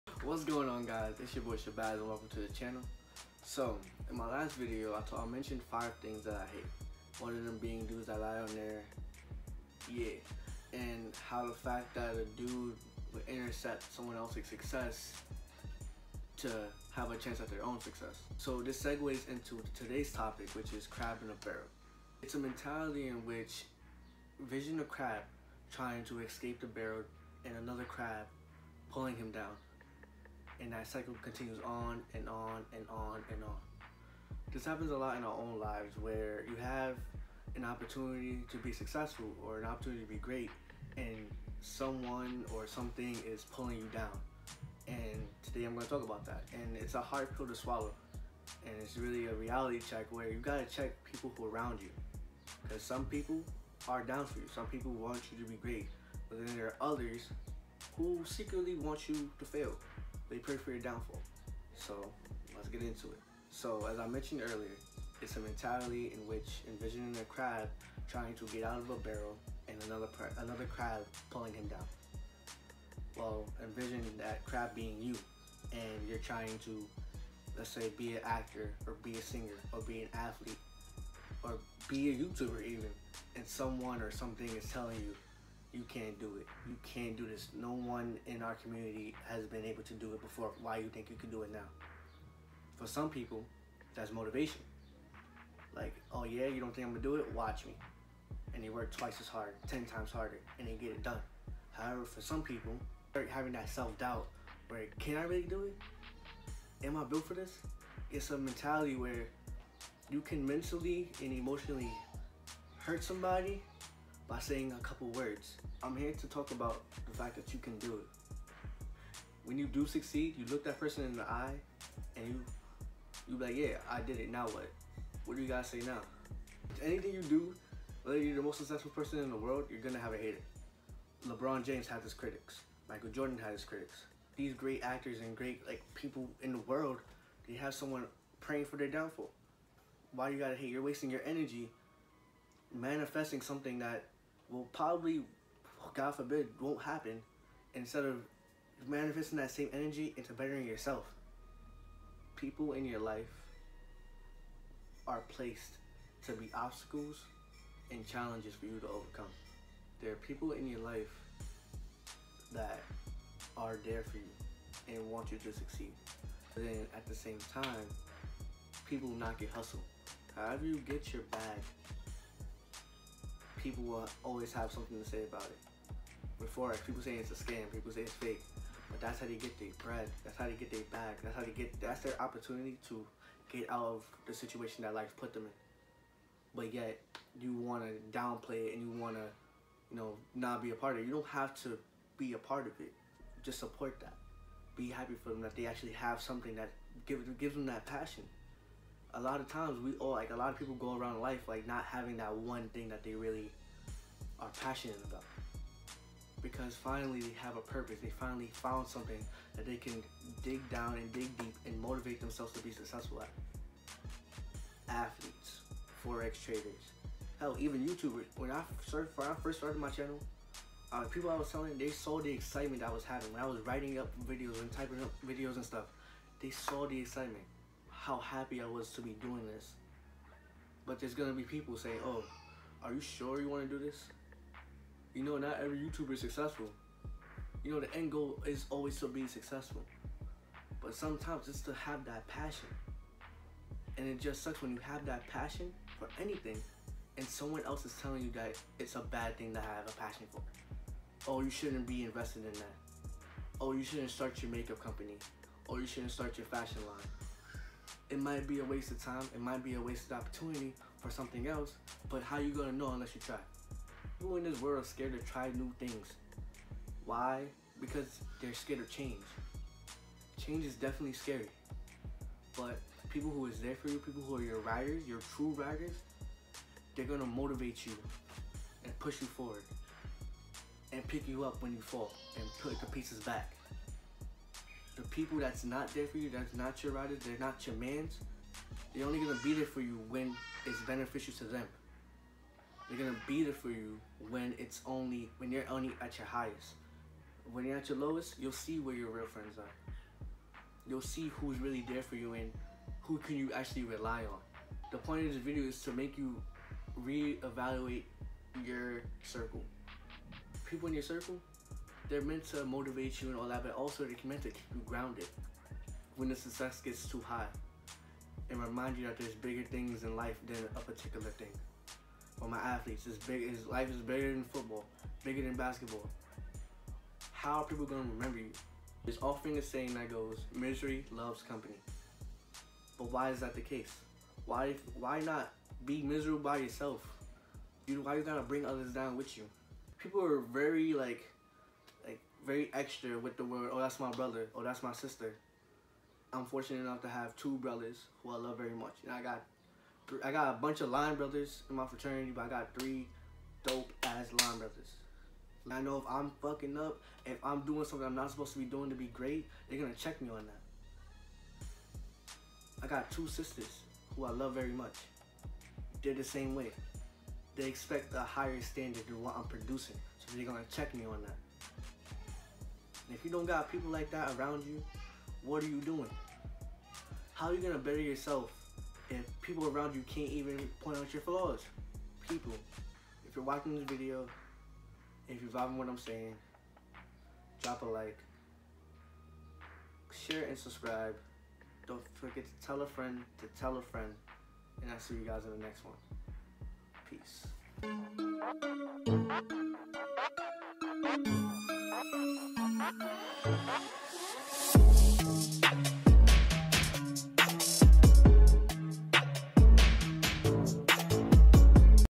Crab in a Barrel | Rant
In this video, I talked about Crab in a Barrel. I recorded this video as more of a rant. Point of this video is to really check those around you and to believe in yourself.